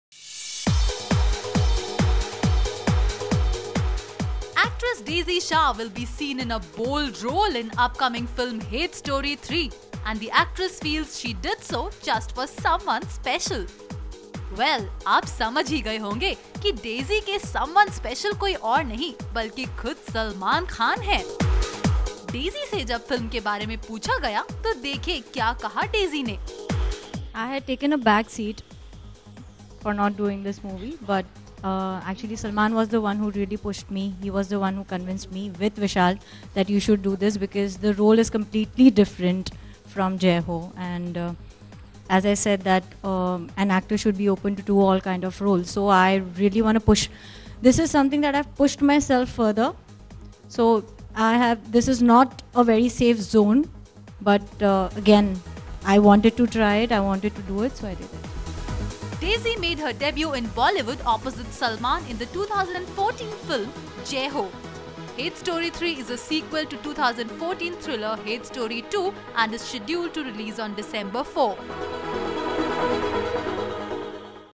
VIDEO: SALMAN KHAN PUSHED ME TO DO 'HATE STORY 3'- DAISY SHAH
Actress Daisy Shah will be seen in a bold role in upcoming film Hate Story 3 and the actress feels she did so just for someone special. Here Daisy Shah disclosed the name of someone special.